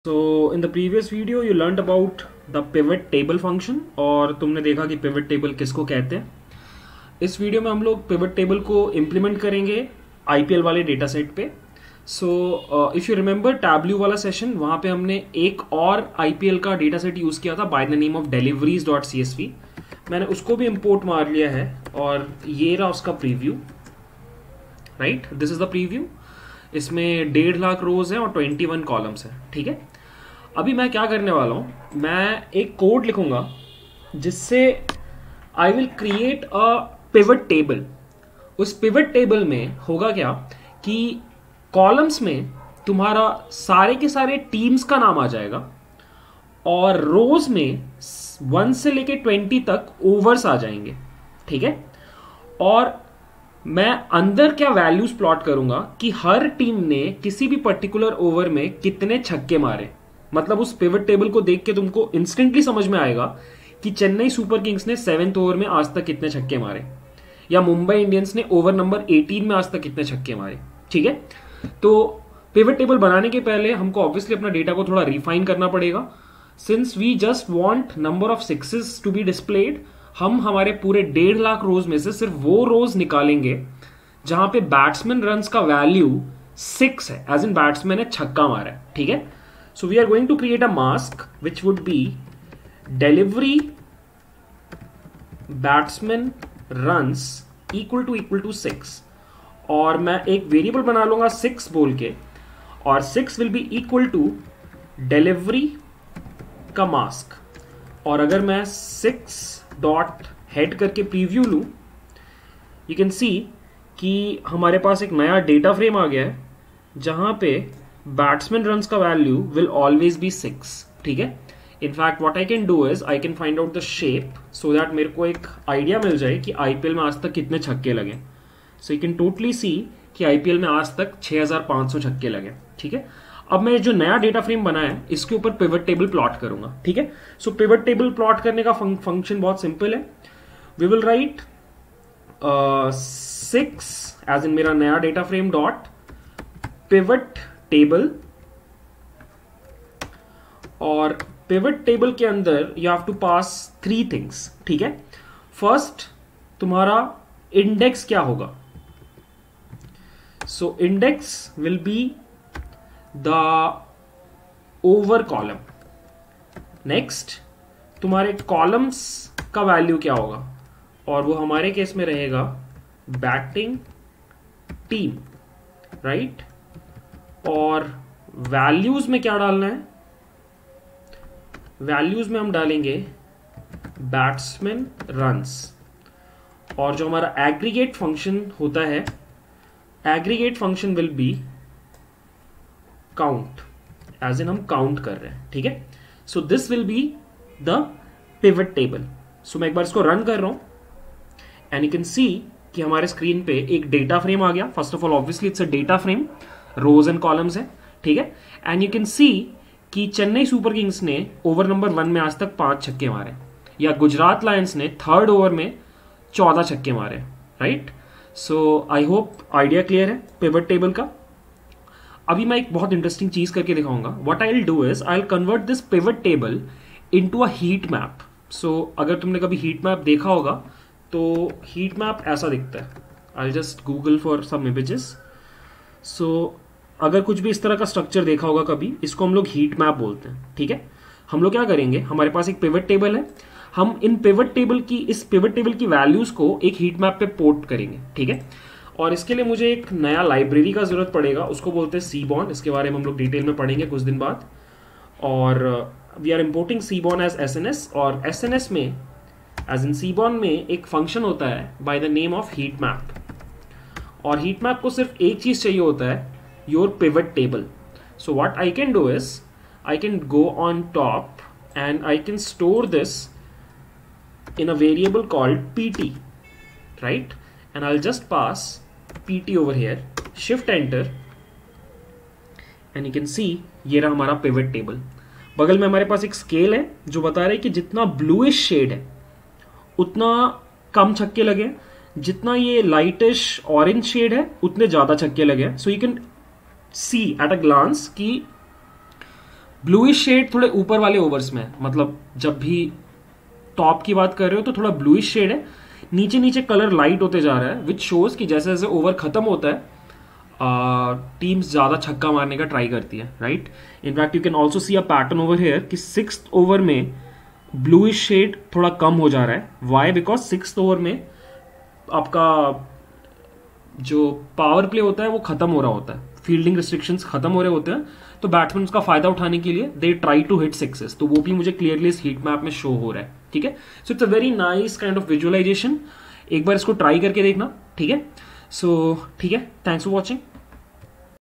Pandas Part 16 - Applying Pivot Table to IPL Dataset
In this video, we will create a pivot table using the IPL dataset and also visualize it using a heatmap About CampusX: